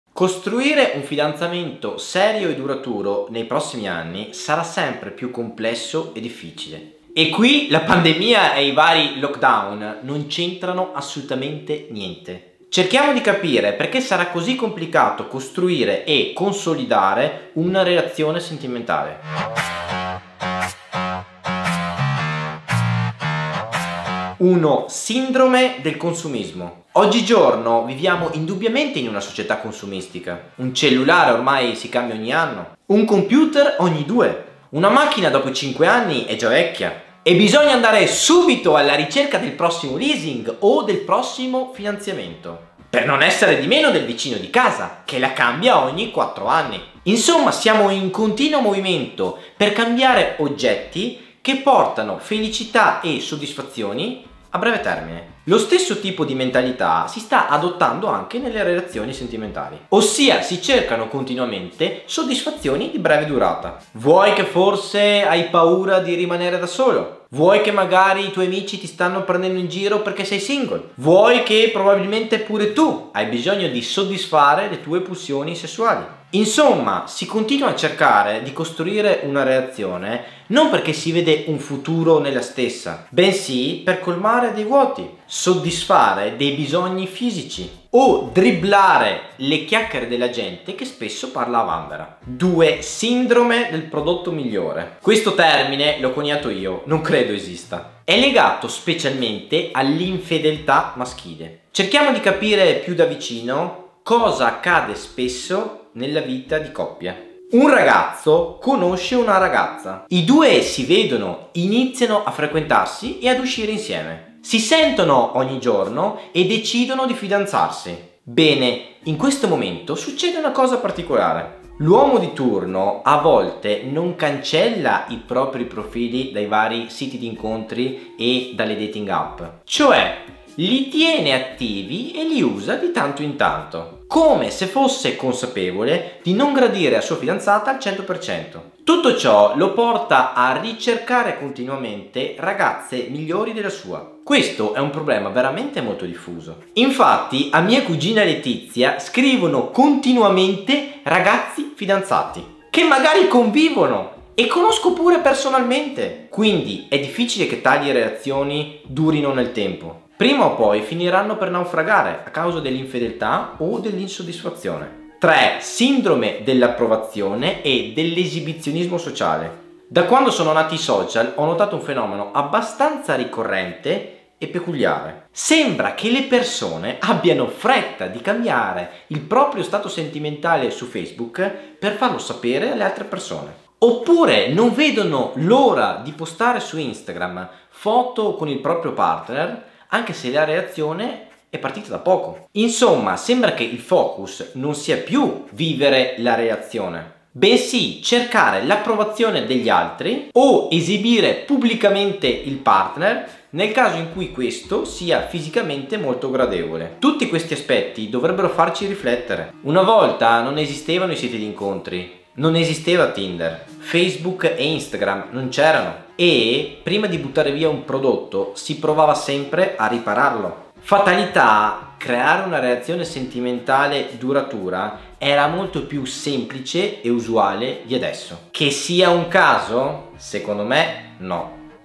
Perché fidanzarsi seriamente sarà sempre più difficile?
È uscita l’Università dell’amore per farti superare il tuo “esame sentimentale” a pieni voti con lode e ottenere la rivalsa sentimentale che meriti

Le relazioni odierne hanno un bel po' di criticità rispetto a quelle del passato...